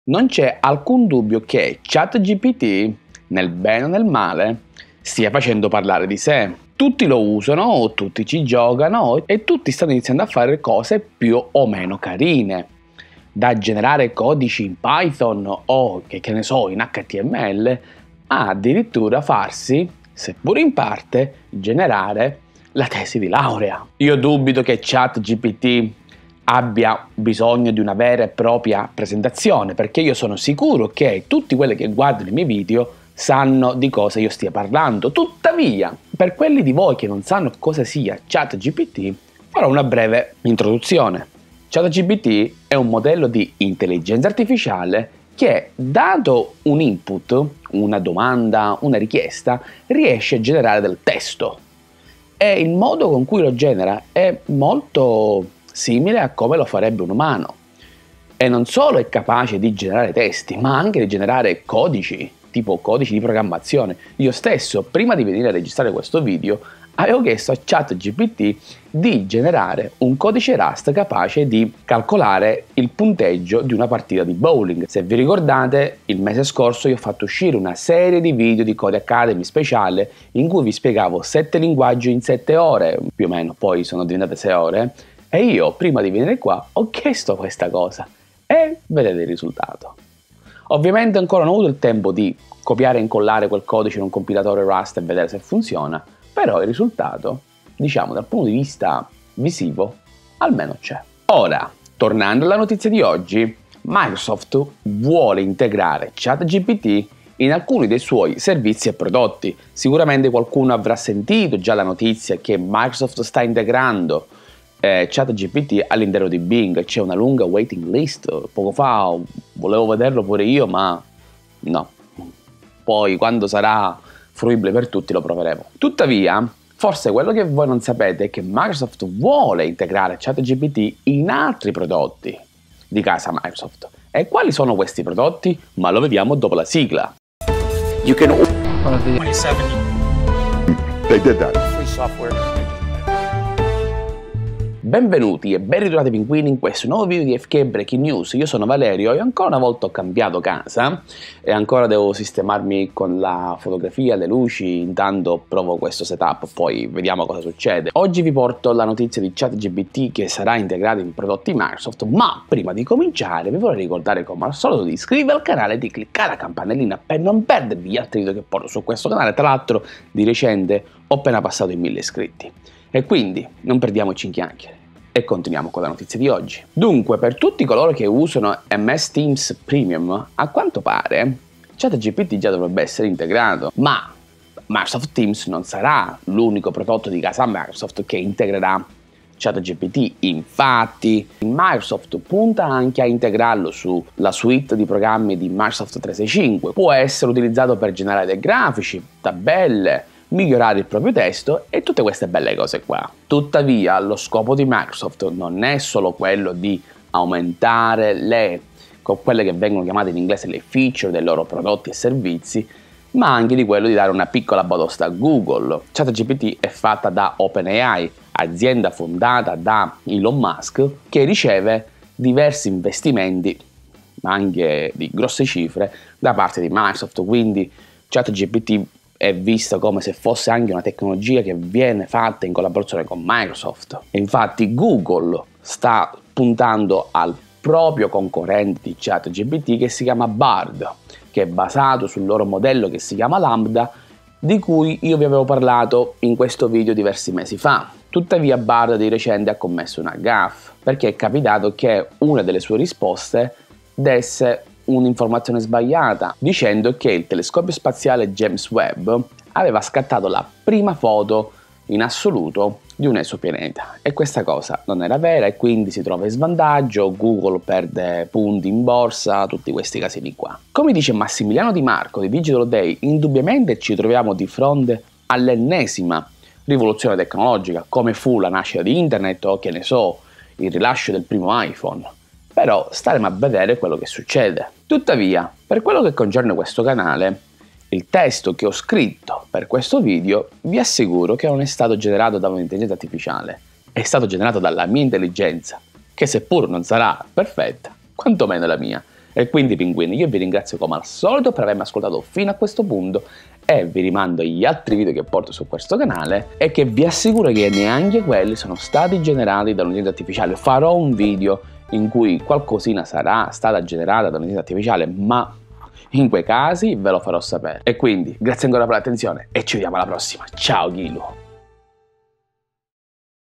ChatGPT integrato su BING e altri prodotti MICROSOFT - AFK BREAKING NEWS
Microsoft vuole integrare ChatGPT su diversi loro prodotti e servizi, quali il loro motore di ricerca Bing, ma anche su MS Teams e nella suite Microsoft 365 (Office). Nel frattempo, Google punta al suo Bard, concorrente di ChatGPT basato sul loro modello LAMBDA.

Video BK-23-005